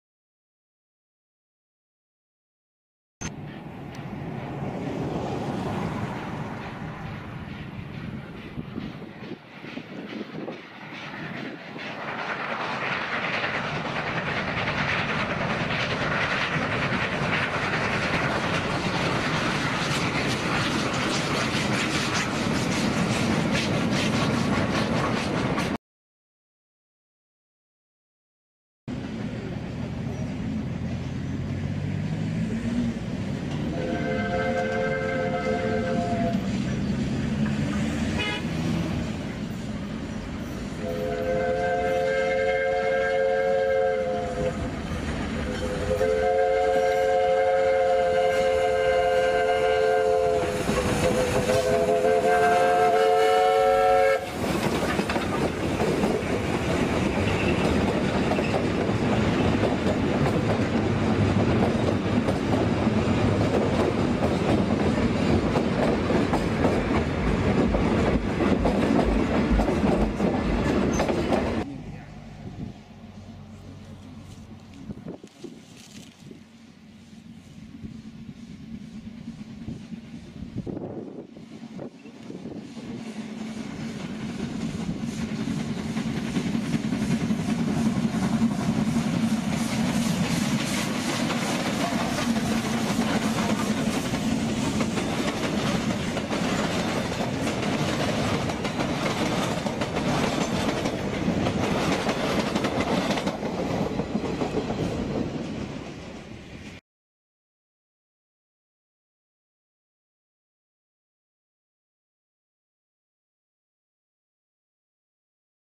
Beyer Garratt 611 On A Test Run To Cement In Preparation For The Mother’s Day Special To Plumtree
LOCOMOTIVE GENERAL PARTICULARS - 16A BEYER GARRATT

Superheated
Tractive Force at 85% B.W.P.: 58,183 lb
Factor of Adhesion at 85% B.W.P & F.W.O: 4.51
Maximum Coupled Axle Load F.W.O: 14.64 tons
Weight on all Coupled Wheels: 117.12 tons
Total Weight of Engine: (Empty): 127.8 tons
Total Weight of Engine: (F.W.O.): 169.18 tons
Weight per foot run over Buffers: 2.05 tons
Water Capacity: Front Tank 3,445 gallons
Water Capacity: Hind Tank 2,155 gallons
Water capacity: Total 5,600 gallons
Coal Capacity: 11 tons
Rigid Wheels Base: 13 ft 1½ ins
Engine Numbers:   620 ~ 649